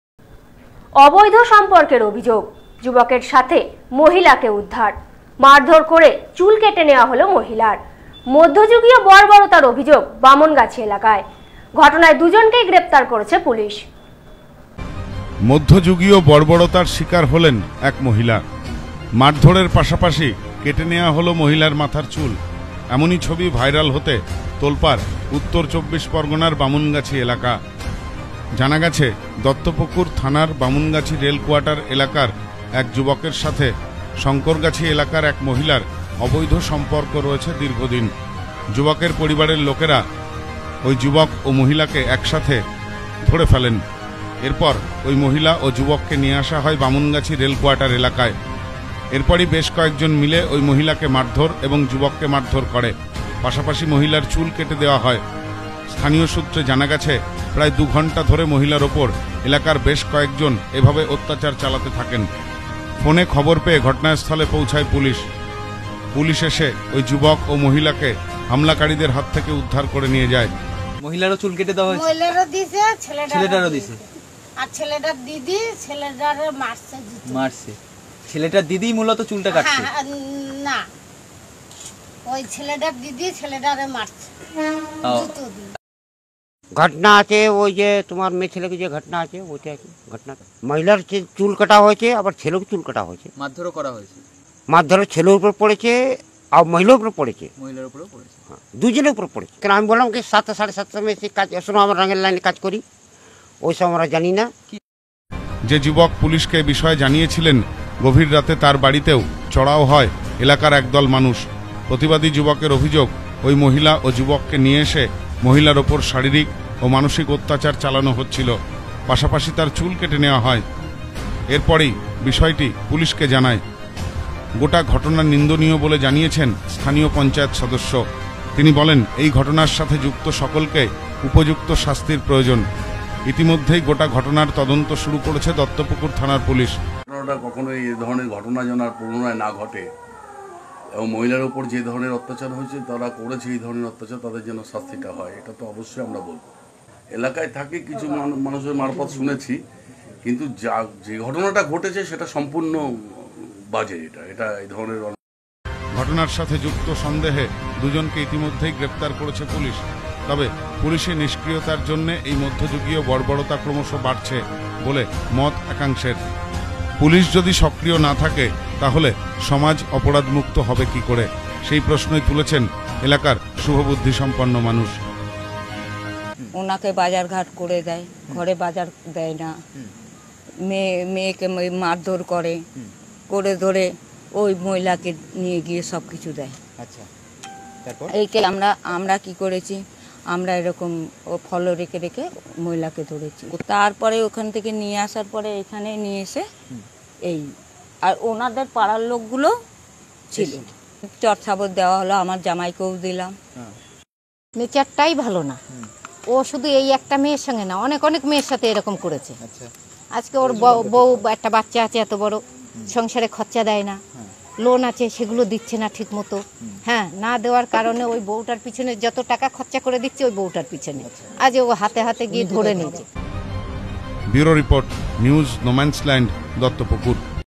অবৈধ সম্পর্কের অভিযোগ। মারধোর করে মাথার চুল কেটে দেওয়া হল মহিলার।
অবৈধ সম্পর্কের অভিযোগ। যুবকের সাথে মহিলাকে উদ্ধার। মারধোর করে মাথার চুল কেটে দেওয়া হল মহিলার। মধ্যযুগীয় বর্বরতার অভিযোগ বামনগাছি এলাকায়। ঘটনায় দুজনকে গ্রেপ্তার করেছে পুলিশ।